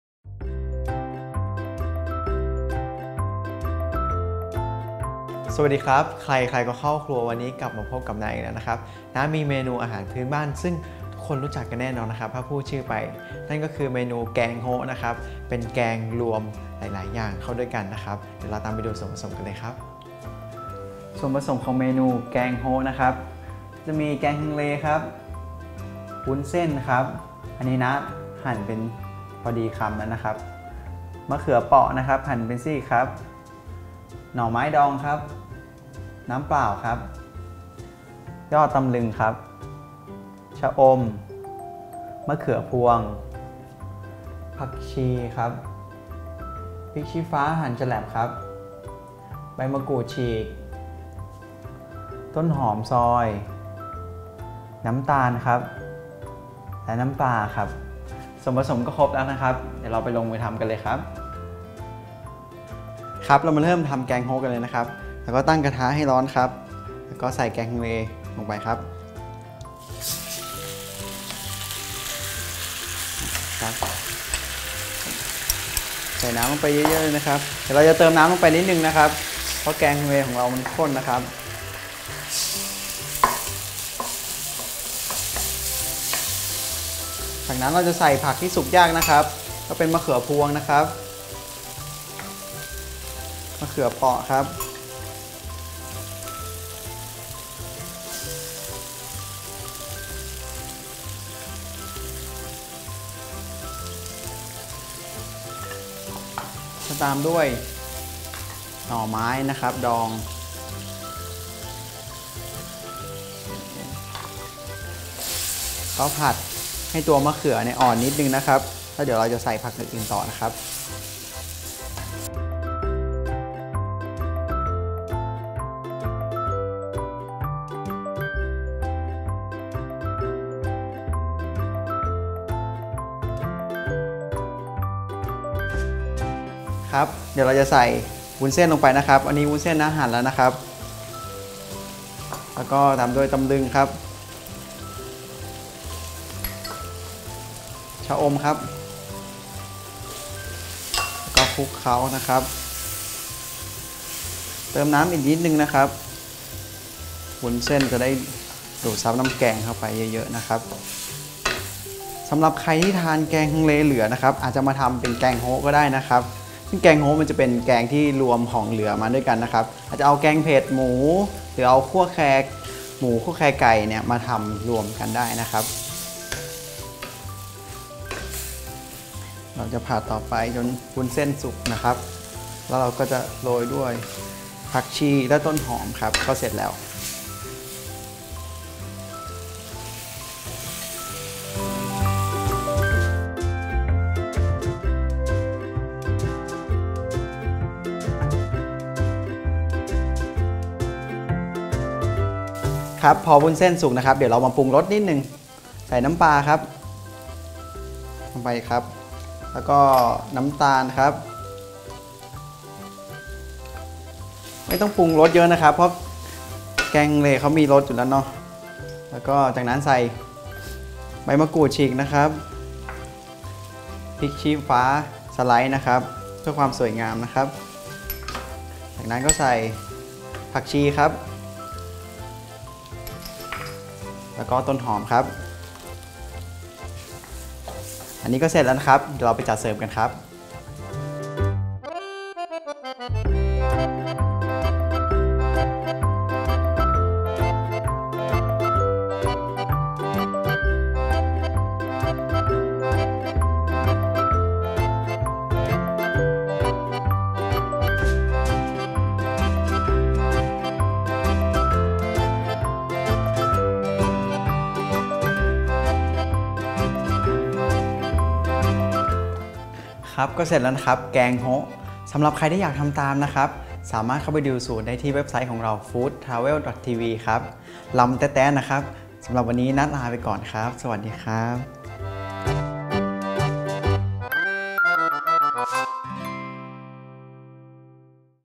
แกงโฮะ อาหารเหนือ ทำง่ายอร่อย | FoodTravel ทำอาหาร
แกงโฮะ แกงโฮ๊ะ Northern Style Mixed Curry นะมีเมนูอาหารพื้นบ้านของภาคเหนือนั้นก็คือแกงโฮ๊ะครับ เป็นเมนูแกงที่นำหลายๆ เมนูมาผสมกัน ใครที่ยังไม่เคยทานนะแนะนำเลยครับ ทานกับข้าวสวยหรือข้าวเหนียวร้อนยิ่งอร่อย

วัตถุดิบแกงโฮ๊ะ Northern Style Mixed Curry

สำหรับ 1 ที่

1. แกงฮังเล 200 กรัม
2.มะเขือเปราะ 2-3 ลูก
3.มะเขือพวง 1/3 ถ้วยตวง
4.หน่อไม้ดอง 1 ถ้วยตวง
5.พริกชี้ฟ้าแดง 1 เม็ด
6.ใบมะกรูดฉีก 6-7 ใบ
7.ต้นหอม 2 ต้น
8.ผักชี 1-2 ต้น
9.วุ้นเส้น 50 กรัม
10.น้ำตาล 1 ช้อนโต๊ะ
11.น้ำปลา 1 ช้อนโต๊ะ
12.ตำลึง 1 ถ้วยตวง
13.ชะอม 1/2 ถ้วยตวง
14.น้ำเปล่า 1 ถ้วยตวง

กระทะ

วิธีทำแกงโฮ๊ะ Northern Style Mixed Curry
1. ตั้งกระทะนำแกงฮังเลใส่ลงไปจากนั้นตามด้วยผักสุกยากอย่าง มะเขือเปราะ มะเขือพวง หน่อไม้ดองผัดให้ผักนิ่ม
2.ใส่วุ้นเส้นลงไป ตามด้วยน้ำเปล่าเล็กน้อย ผัดจนให้เส้นเริ่มนิ่ม
3.ปรุงรสด้วย น้ำปลาและน้ำตาล จากนั้นใส่ชะอม ตำลึง ใบมะกรูด พริกชี้ฟ้าแดง ต้นหอม ผักชีลงไปผัดต่อจนผักสุก จากนั้นจึงตักเสิร์ฟ

Ingredient Cooking

1-2 serve

1. Hunglay curry 200 g.
2. Thai eggplant 2-3
3. Turkey berry 1/3 cup.
4. Sour bamboo shoots 1 cup.
5. Red chili 1
6. Kaffir lime leave 6-7
7. Spring onion 2
8. Coriander 1-2
9. Vermicelli 50 g.
10. Sugar 1 tbsp.
11. Fish sauce 1 tbsp.
12. Ivy gourd 1 cup.
13. Climbing wattle 1/2 cup.
14. Water 1 cup.